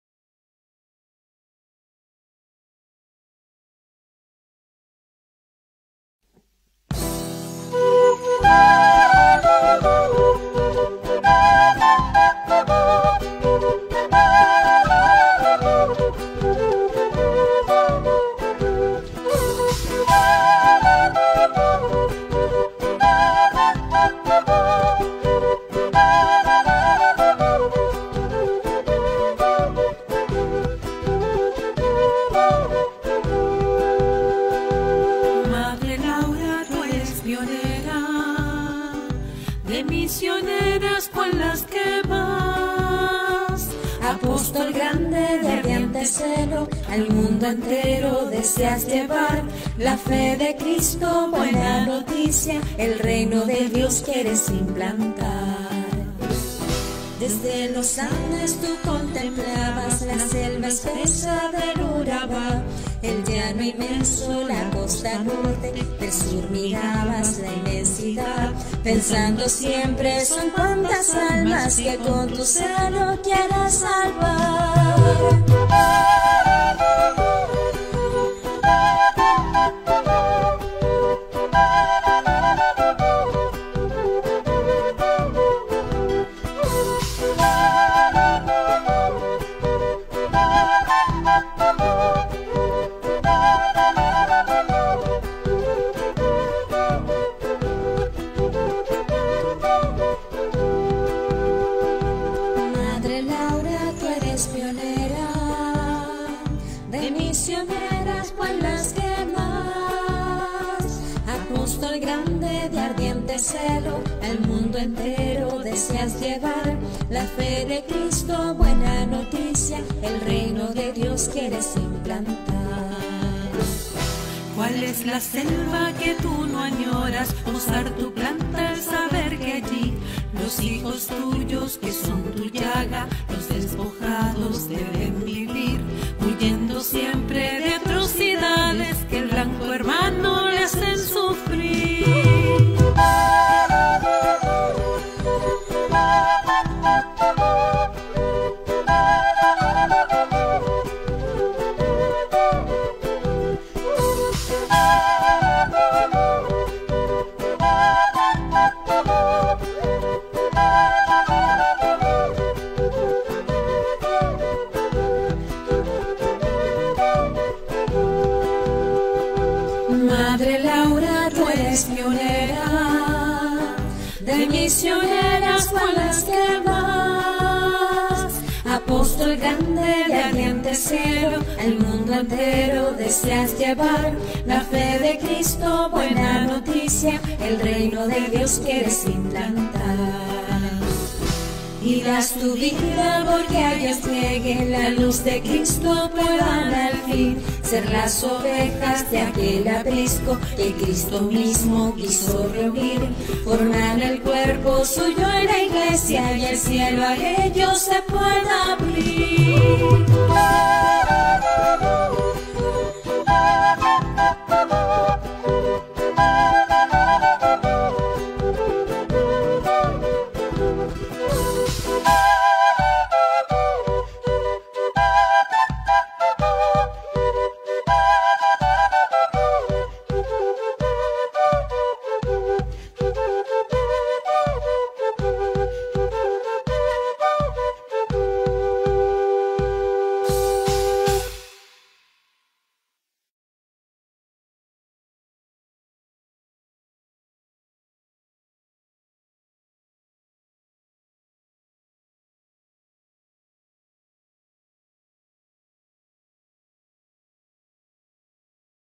Saludo a Santa Laura Montoya Upegui
En el día de la celebración de Santa Laura Montoya Upegui, saludamos con cariño a la Congregación de Misioneras Lauritas.